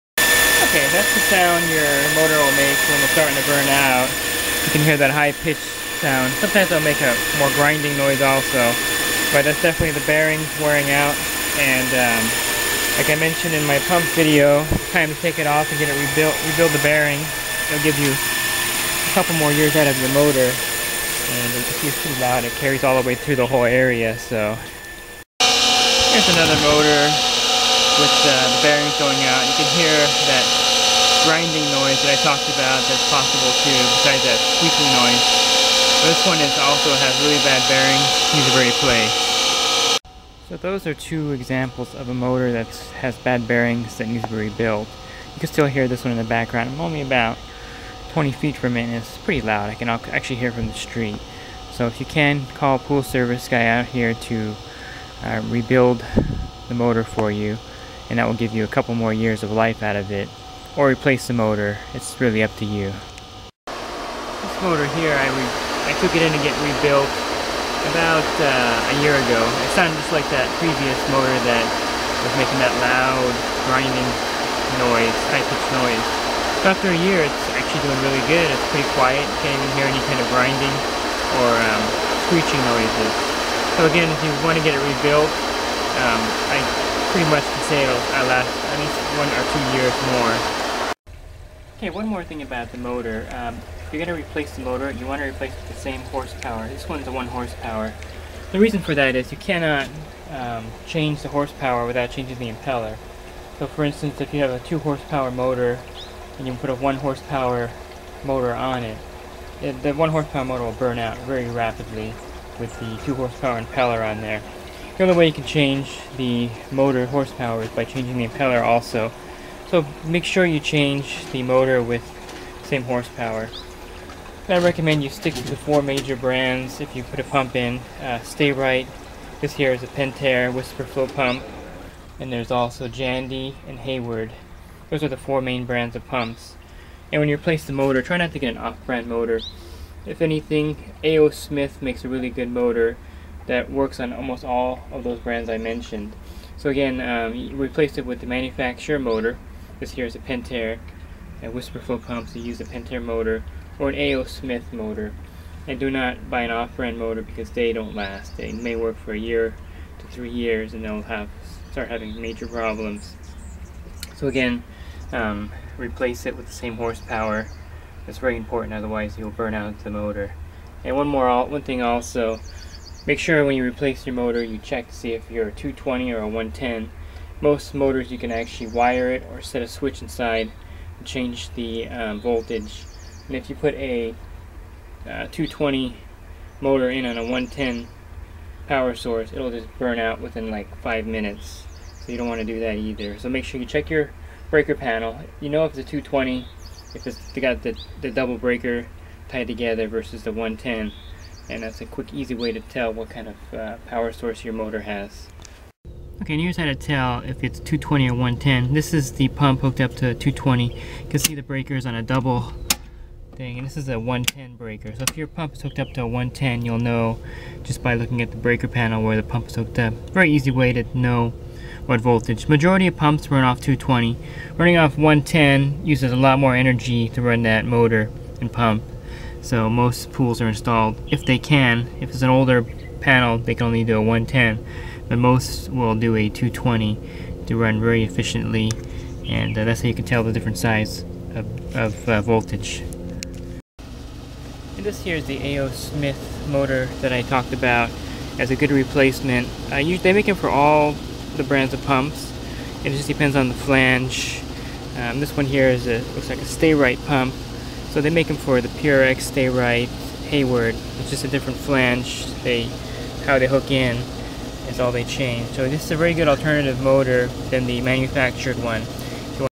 Pool Motor Tips and Walk Thru
NOTES:
Motor problems: If your motor starts to make a loud whining are grinding noise chances are good the bearings are going out. Call a pool professional, or pull it off yourself and have the bearings rebuilt before it burns out. I charge $120-$140. A new motor will run you $400. This will extend the motor life by 2 or 3 years. A great money saving tip for you.

If your house circuit breaker is tripped when you turn the motor on, chances are it is burned out and needs to be replaced (bummer).

If you turn on the pool timer and the motor struggles to start, but then starts after a few seconds to a few minutes, the starter capacitor is going out. Get it replaced and the motor will run fine.

Last, if you see water leaking  from the middle of the motor, behind the bolts or clap that hold it to the pump (usually a pretty sizable leak, not a drip leak) the pump seal has failed. It's a $5.00 part and easy to replace.  If it goes unfixed it will rust out the motor and burn it out. Remove your motor and take it in if you are not handy, or call a respected pool pro to change out the pump seal for you.

IMPELLER JAM: SOMETIMES A TWIG OR ROCK WILL JAM THE IMPELLER. THE MOTOR WILL COME ON, THEN BUZZ AND SHUT OFF. CHECK THE IMPELLER TO SEE IF IT SPINS FREELY, IF NOT REACH IN CAREFULLY (WITH EQUIPMENT TOTALLY OFF AT THE BREAKER) AND REMOVE DEBRIS.